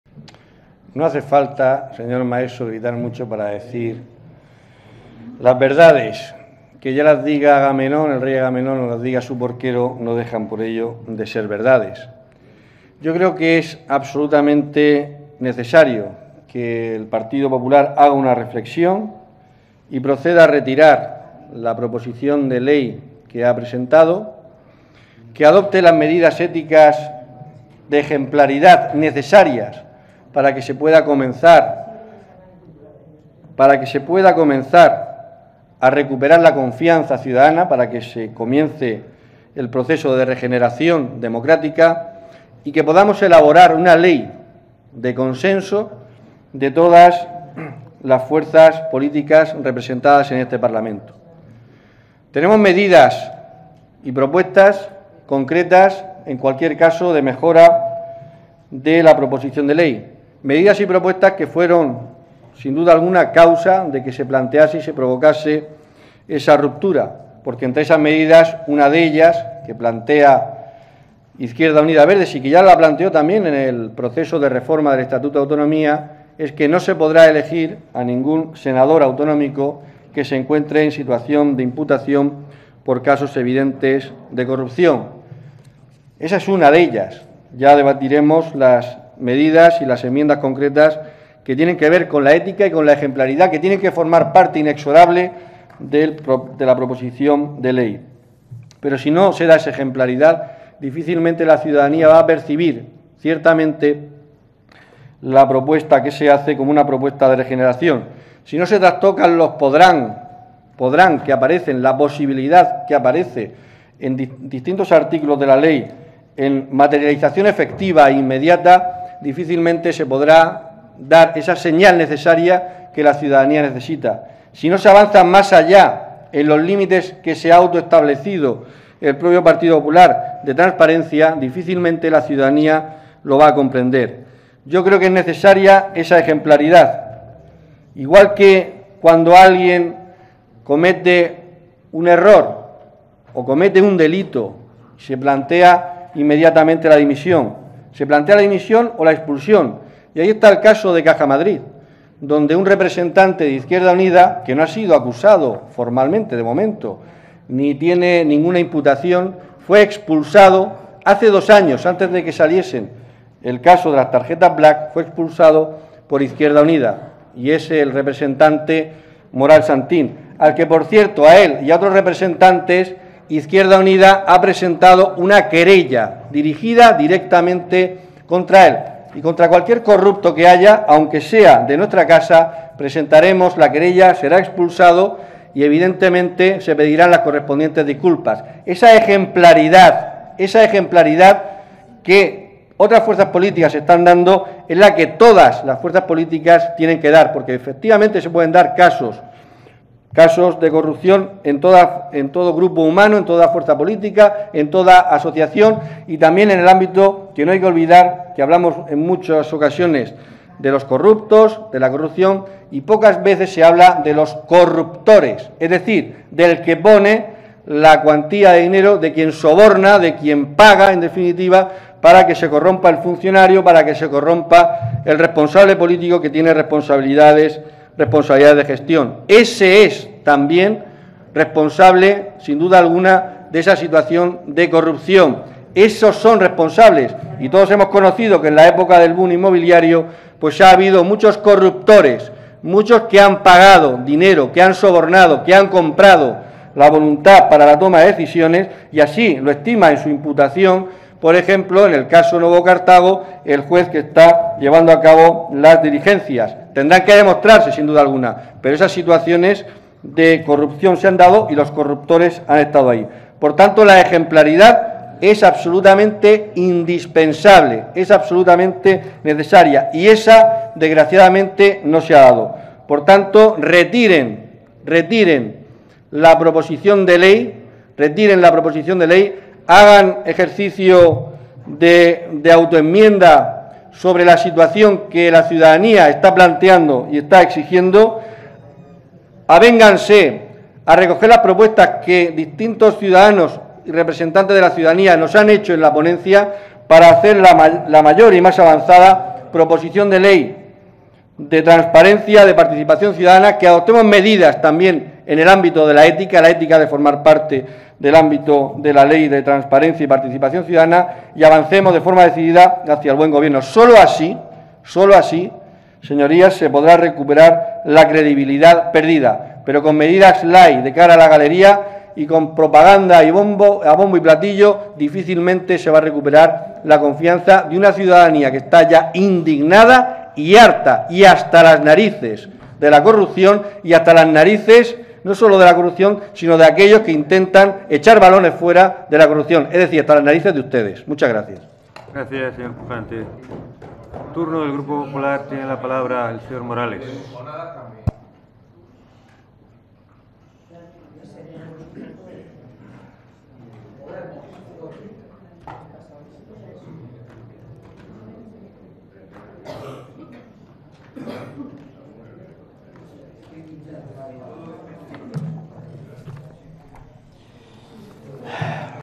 5 PUJANTE DIEKMANN JOSE ANTONIO
DEBATE DE TOTALIDAD DE LA PROPOSICIÓN DE LEY DE TRANSPARENCIA Y PARTICIPACIÓN CIUDADANA DE LA COMUNIDAD AUTÓNOMA DE LA REGIÓN DE MURCIA, FORMULADA POR EL GP. POPULAR.